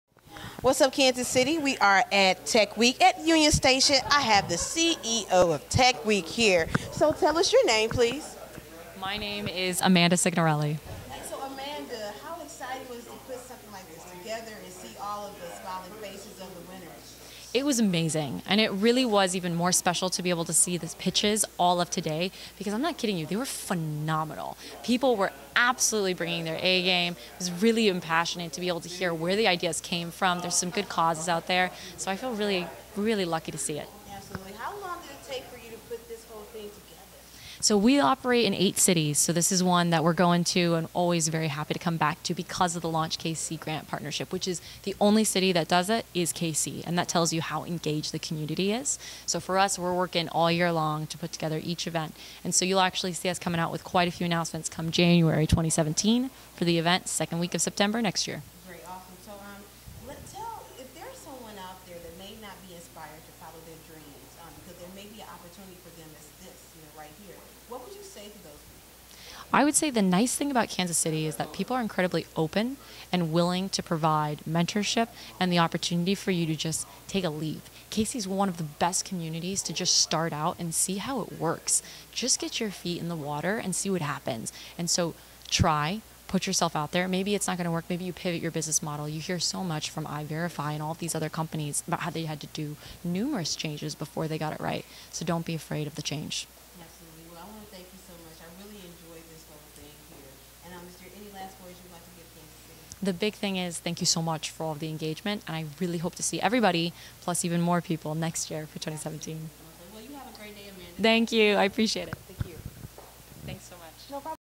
Interview with Amanda Signorelli Ceo of KC TechWeek
Cascade Media Group covering TechWeek events at Union Station.Interview with Amanda Signorelli Ceo of KC TechWeek